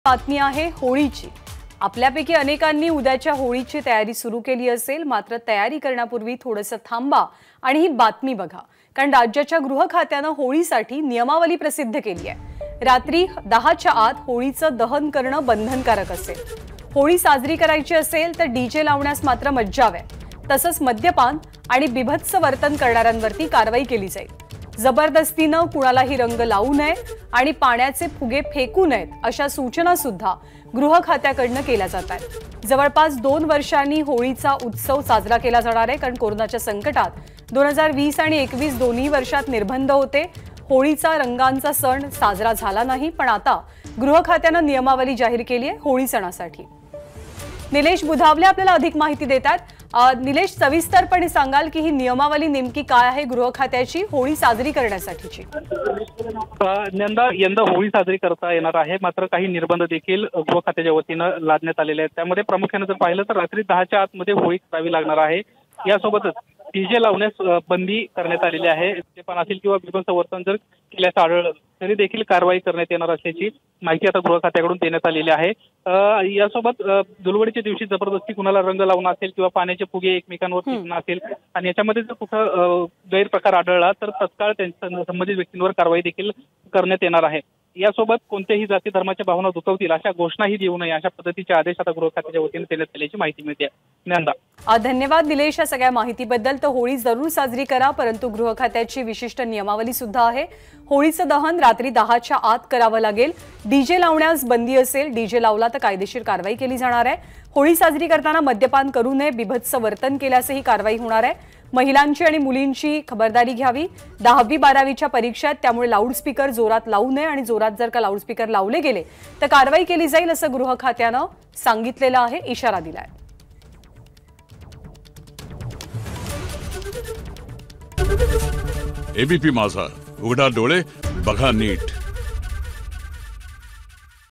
Holi Rules : होळीसाठी गृहखात्याची नियमावली प्रसिध्द, रात्री दहाच्या आत होळी दहन करावी लागणार
राज्याच्या गृह खात्यानं होळीसाठी नियमावली प्रसिद्ध केली आहे.. रात्री दहाच्या आत होळीचं दहन करणं बंधनकारक असणार आहे..  होळी साजरी करताना डीजे लावण्यास बंदी असेल.. तसंच मद्यपान आणि बीभत्स वर्तन करणाऱ्यांवर कारवाई करण्यात येणार आहे..  जबरदस्तीनं रंग लावू नये आणि पाण्याचे फुगे फेकू नये अशा गृह खात्यानं सुचना दिल्यात.